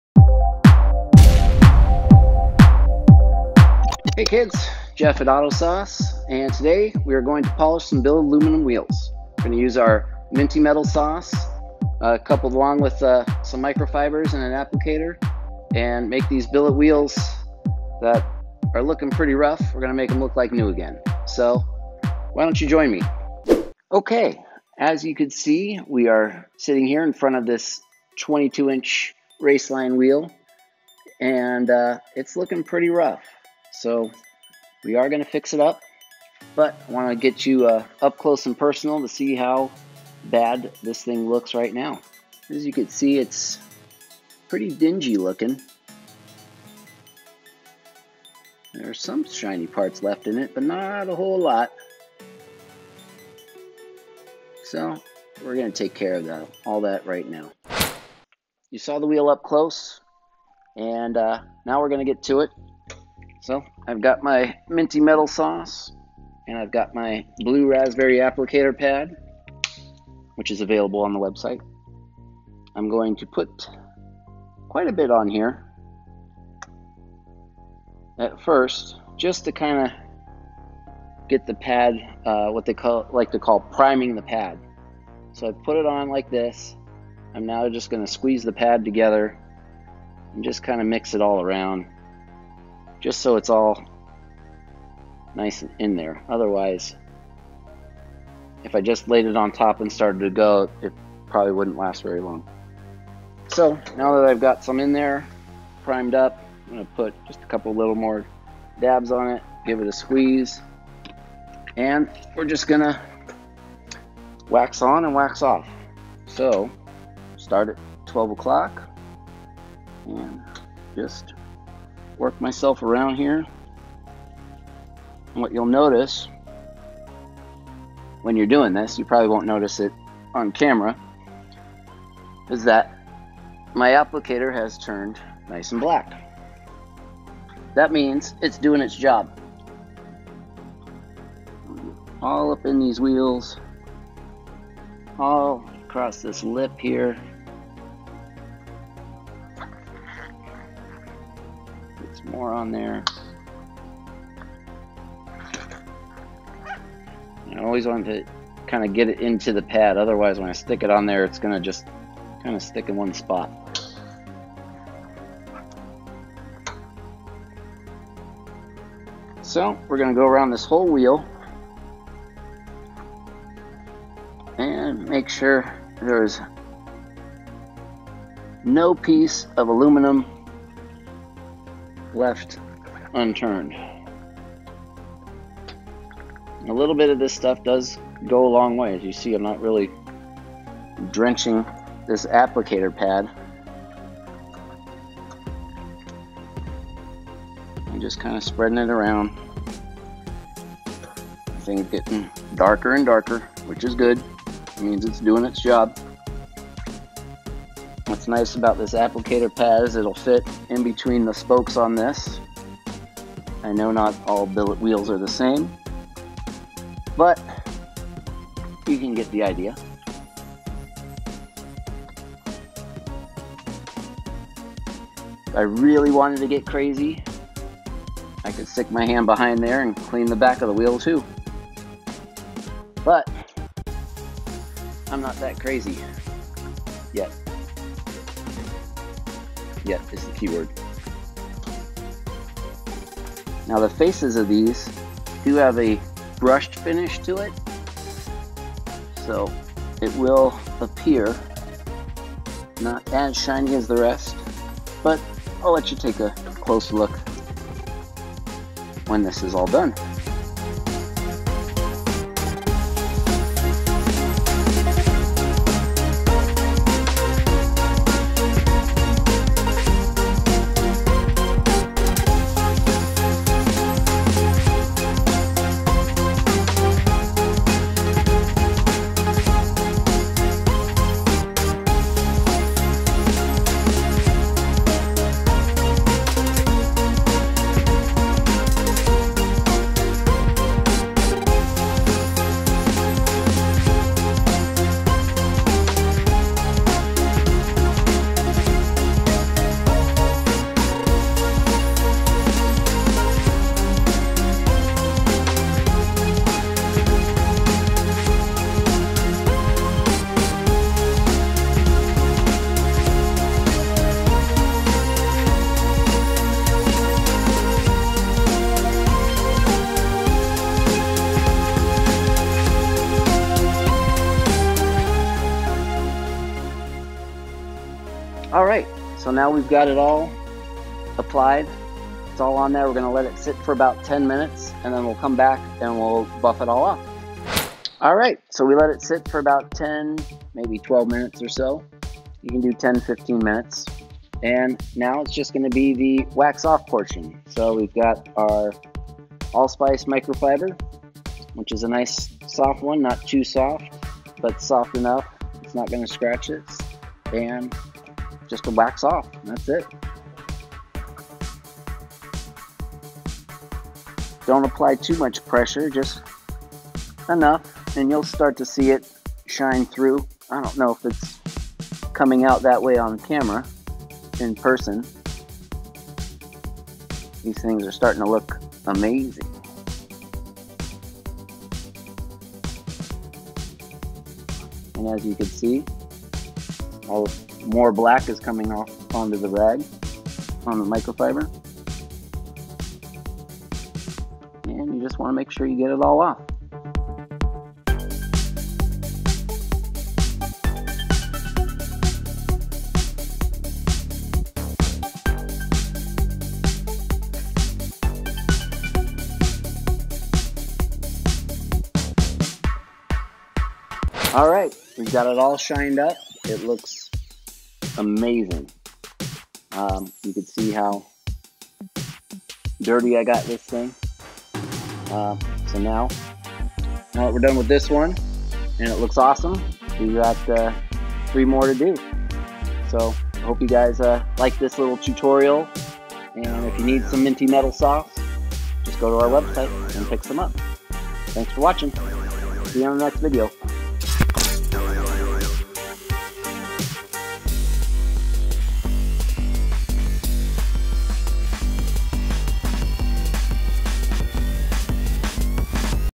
Auto Sauce - How to Use Minty Metal Sauce On Wheels
Jeff here from Auto Sauce!
Today we will be using minty metal sauce to clean the wheels on this vehicle!

FOLLOW US MORE!

Address:
15882 Manufacture Lane